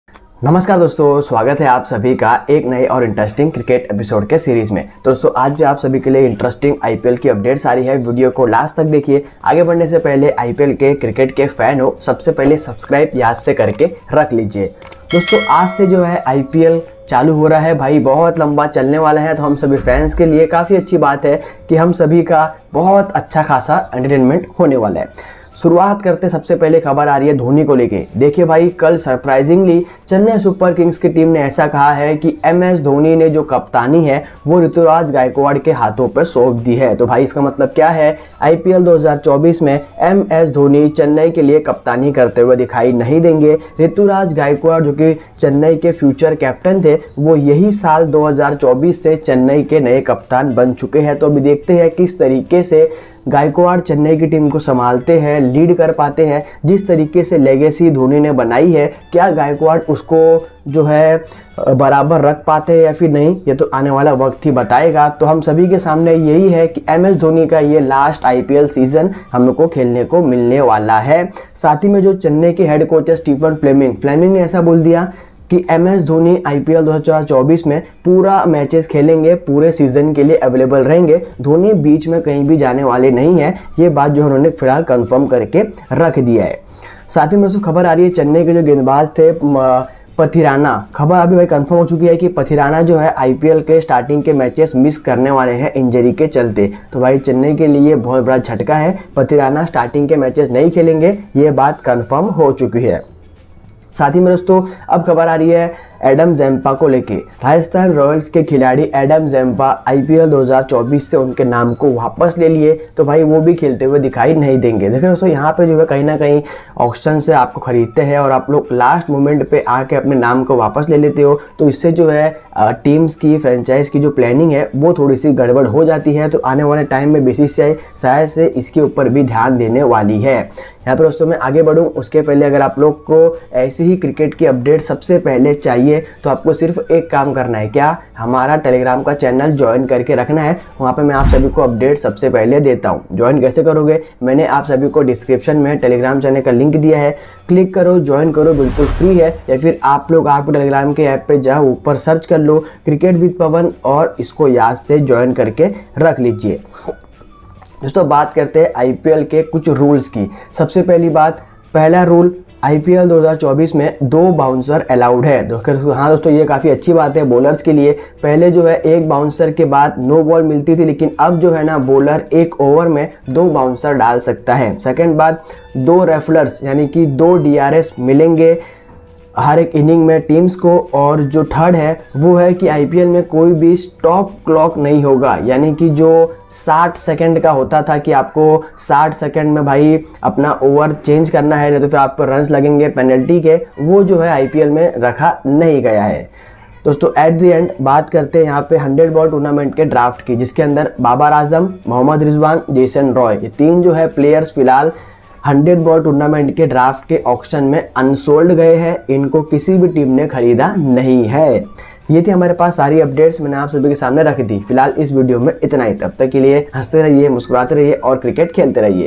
IPL 2024 CSK New Captain, Dhoni IPL Retirement, IPL 2024 3 New Rules
News Covered: IPL 2024 CSK New Captain, Dhoni IPL Retirement, IPL 2024 3 New Rules